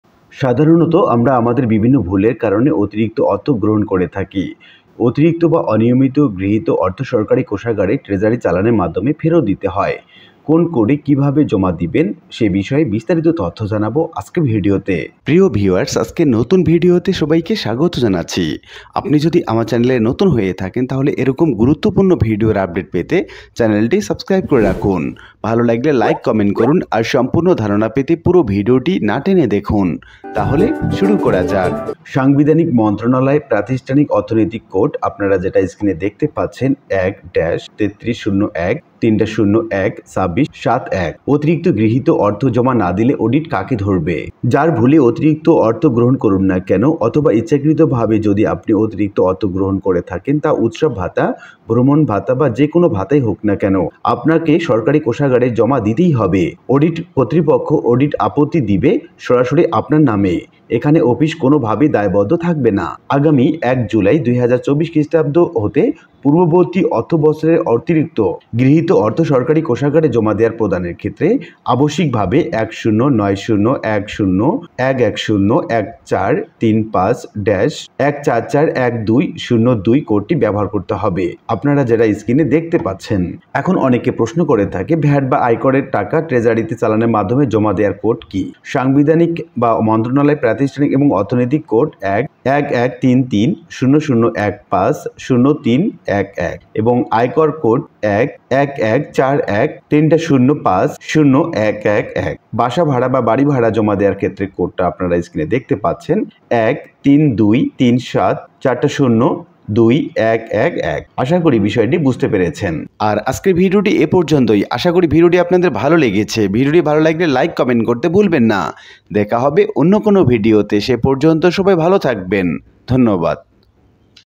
অতিরিক্ত গৃহীত অর্থ ১ জুলাই/২০২৪ থেকে জমা করুন নতুন কোডে! ভ্যাট/আয়কর/বাসা ভাড়া দেয়ার নতুন কোড প্রকাশ
অতিরিক্ত গ্রহীত অর্থ কোন কোডে ফেরত দিবো এটা নিয়ে দ্বিধা দন্ডে পড়ে যাই। বেশির ভাগ ক্ষেত্রে বেতন বা বাড়ি ভাড়া অতিরিক্ত গ্রহণ করা হয়। এটি পরবর্তীতে সরকারি কোষাগারে ফেরত দেওয়ার প্রয়োজন পড়ে।
        প্রশ্নোত্তর পর্ব:
            অতিরিক্ত গৃহীত অর্থ জমা না দিয়ে অডিট কাকে ধরবে?

প্রশ্নোত্তর পর্ব:

প্রশ্ন: ভ্যাট ও আয়করের টাকা ট্রেজারীতে চালানের মাধ্যমে জমার কোড কি কি?

উত্তর:  সাংবিধানিক-মন্ত্রণালয়-প্রাতিষ্ঠানিক-অর্থনৈতিক কোড

    ভ্যাট কোড:              ১-১১৩৩-০০১৫-০৩১১
    আয়কর কোড:         ১-১১৪১-০০০৫-০১১১
    বাসা ভাড়া বা বাড়ি ভাড়া জমা দেয়ার কোড: ১-৩২৩৭-০০০০-২১১১

# সাধারণত আমরা আমাদের বিভিন্ন ভুলের কারণে অতিরিক্ত অর্থ গ্রহণ করে থাকি। অতিরিক্ত বা অনিয়মিতভাবে গৃহীত অর্থ সরকারি কোষাগারে ট্রেজারি চালানের মাধ্যমে ফেরত দিতে হয়। কোন কোডে কিভাবে জমা দিবে নিচে দেওয়া হলো।

যার ভুলেই অতিরিক্ত গ্রহণ করে থাকেন না কেন। অথবা ইচ্ছাকৃত ভাবেও যদি আপনি অতিরিক্ত অর্থ গ্রহণ করে থাকেন তা উৎসব ভাতা, ভ্রমণ ভাতা বা যে কোন ভাতাই হোক তা আপনাকে সরকারি কোষাগারে জমা দিতে হবে। অডিট কর্তৃপক্ষ অডিট আপত্তি দিবে সরাসরি আপনার নামে, এখানে অফিস কোন ভাবেই দায়বদ্ধ থাকে না।
আগামী ১ জুলাই ২০২৪ তারিখ হতে পূর্ববর্তী অর্থ বছরের অতিরিক্ত গৃহীত অর্থ সরকারি কোষাগারে জমা প্রদানের ক্ষেত্রে আবশ্যিকভাবে ১০৯০১০১১০১৪৩৫-১৪৪১২০২ কোডটি ব্যবহার করতে হবে।